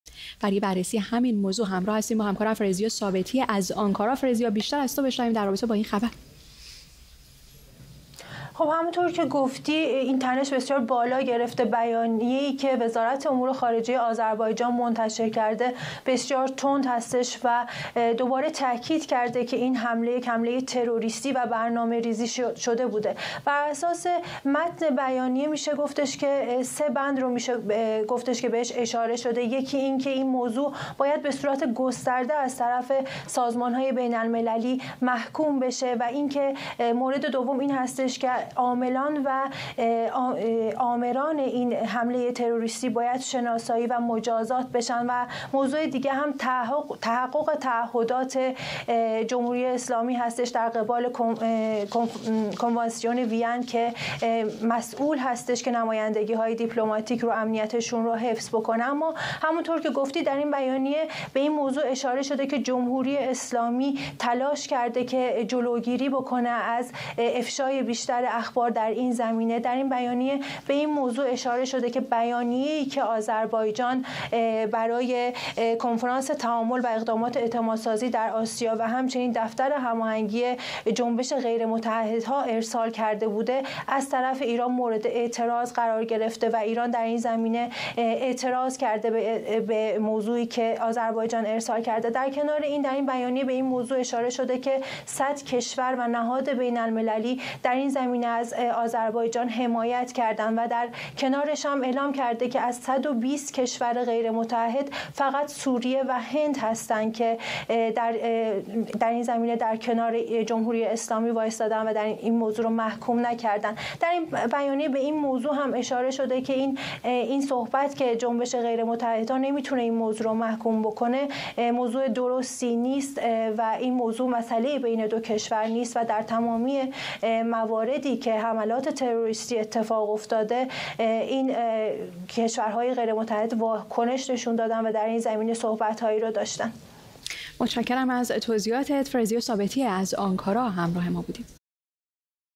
باکو، تهران را به کارشکنی متهم کرد؛ بازداشت ۳۹ نفر به اتهام جاسوسی برای جمهوری اسلامی
گفت‌وگو با فرزیا ثابتی، ایران‌اینترنشنال